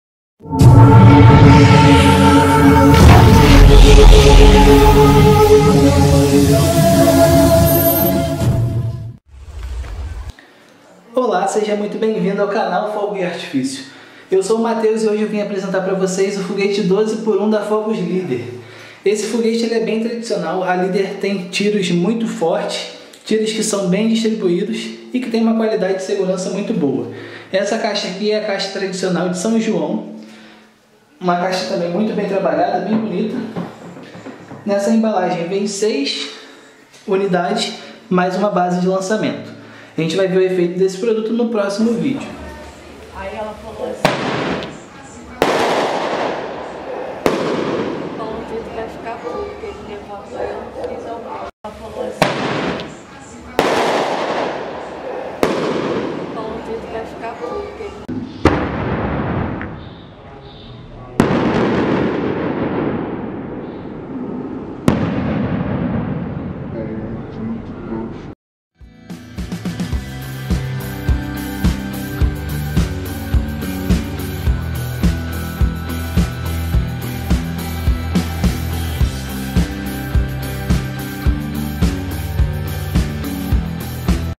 FOGUETE 12X1 - FOGOS LÍDER
Seja bem-vindo ao Maior Canal de Pirotecnia do BRASIL!

No vídeo de hoje apresentamos o tradicional Foguete 12x1 da Fogos Líder.

SOMOS O MAIOR CANAL DO BRASIL ESPECIALIZADO EM FOGOS DE ARTIFÍCIO.